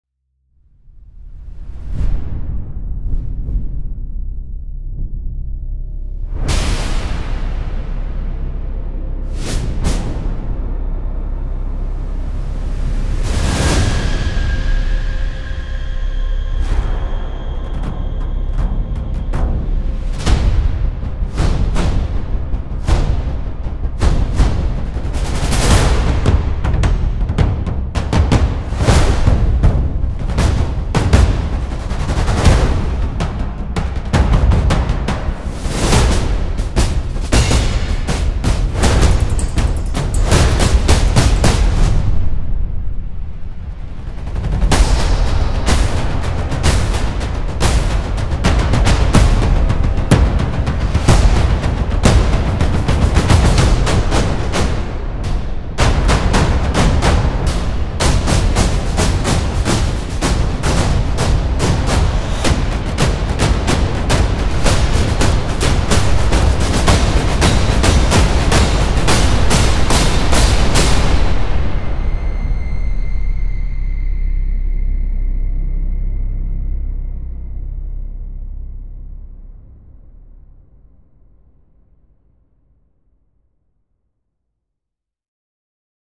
Strikeforce Trailer
STRIKEFORCE is a brand new cinematic percussion library from Laboratory Audio. Massive ensemble drums with lots of character, ready to blow up the movie screen. The uniquely designed STRIKEFORCE user interface makes it a breeze to create BIG, natural sounding, cinematic drum beats.

All percussions in this track are created with STRIKEFORCE

Music composed by Thor Laewe